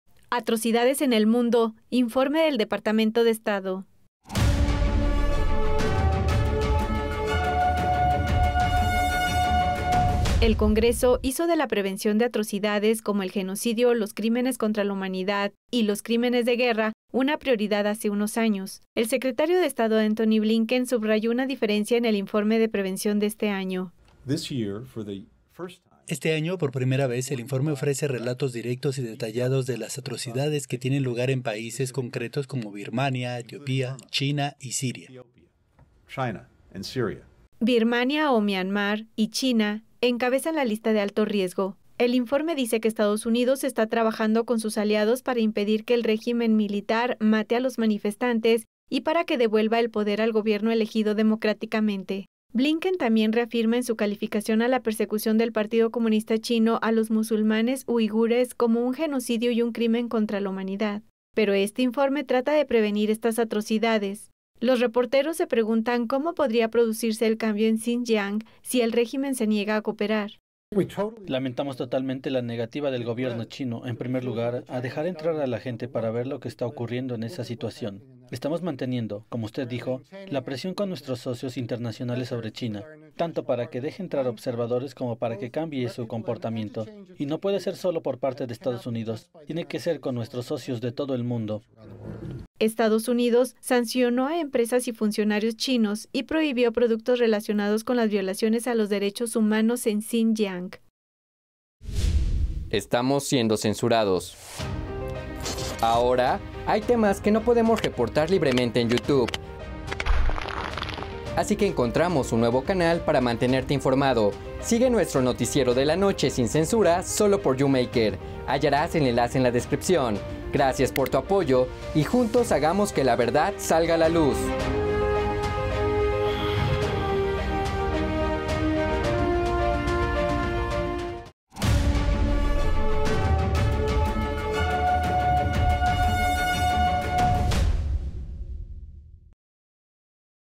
Atrocidades en el mundo: informe del Dpto. de Estado
Se publicó el último informe de Estados Unidos sobre las atrocidades en el mundo. China y Birmania encabezan la lista de alto riesgo del Departamento de Estado. Esta es la información.

💎  ¿Defenderá EE.UU. a Taiwán en caso de que estalle una guerra con China?